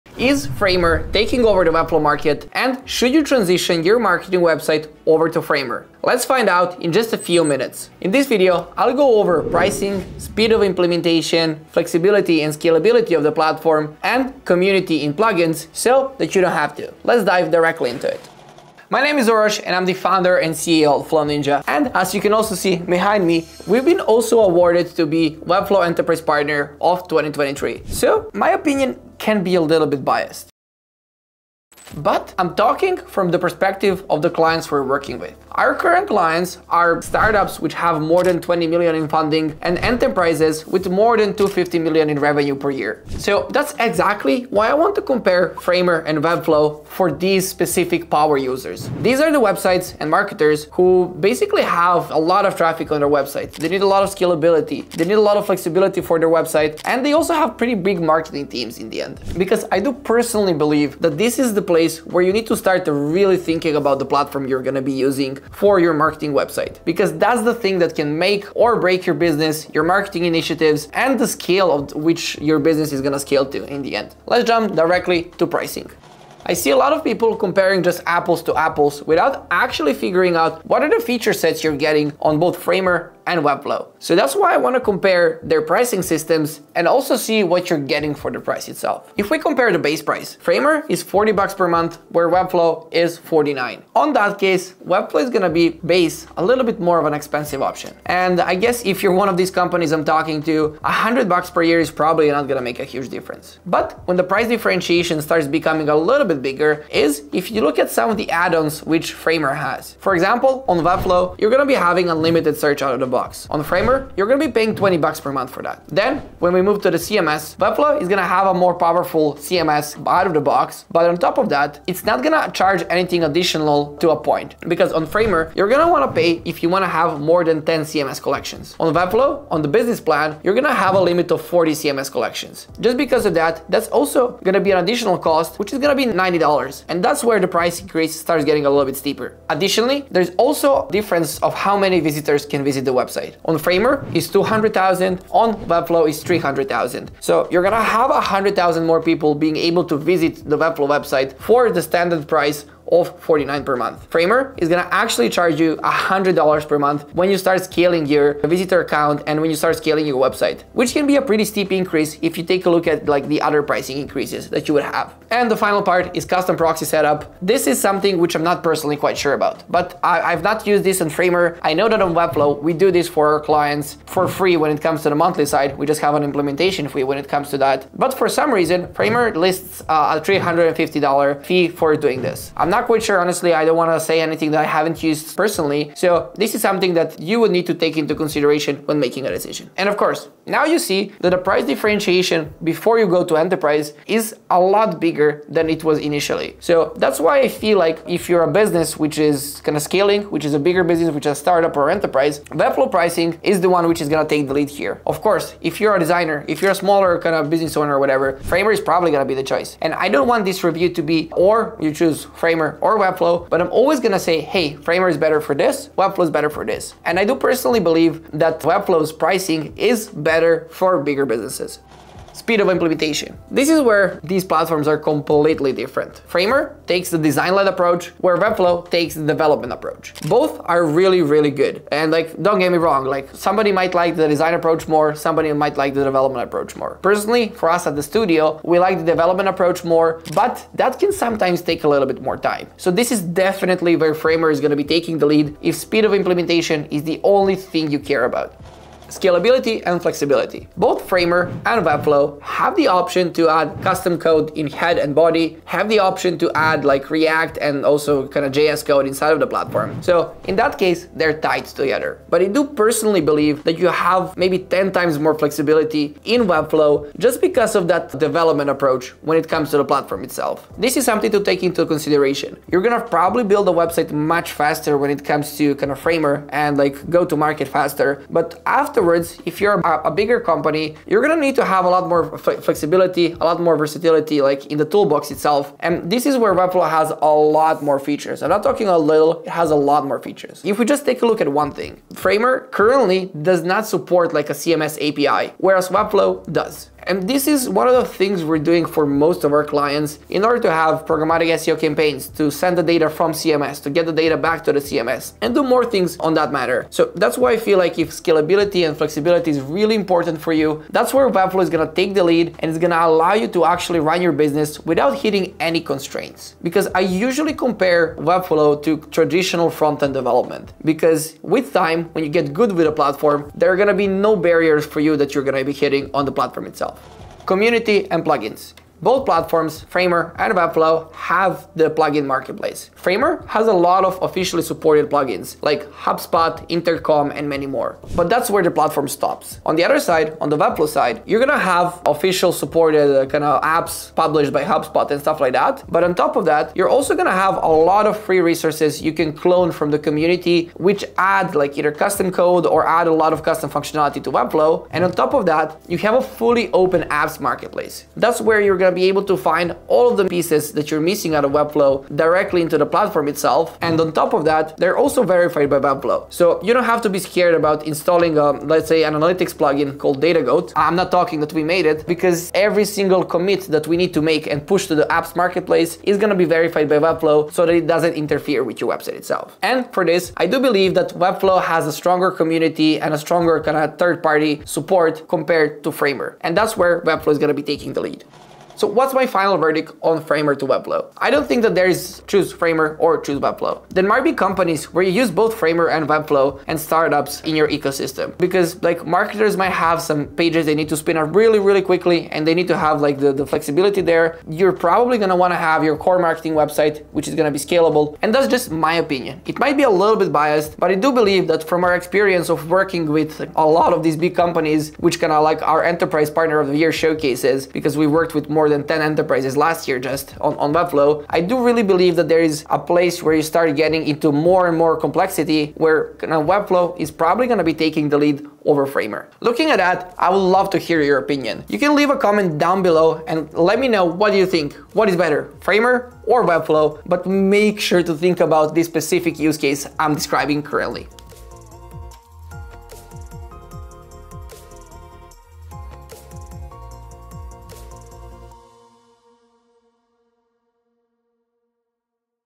Webflow vs Framer THE ULTIMATE Choice for 2025
In this video, I will talk about how Framer and Webflow are compared as website-building options.

Have a project in mind? Book a free consulting call with our team